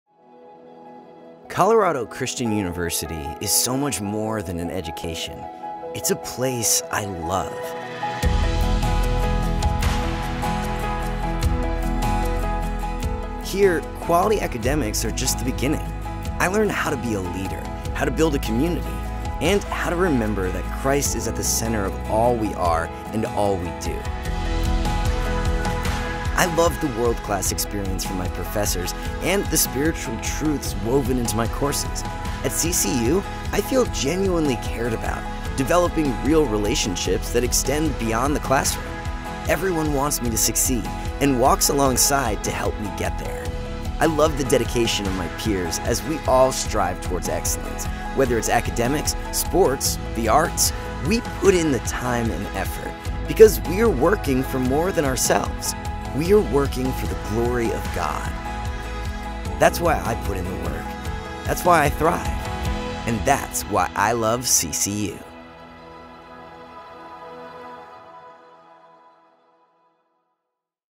More Than an Education | Colorado Christian University
Quality academics are just the beginning at CCU. Learn and embrace how to be a leader, build a community, and live a life for Christ through are integration of faith and learning all centered on Jesus Christ, the Living Word, the core of all that CCU is and does.

Apply at ccu.edu/apply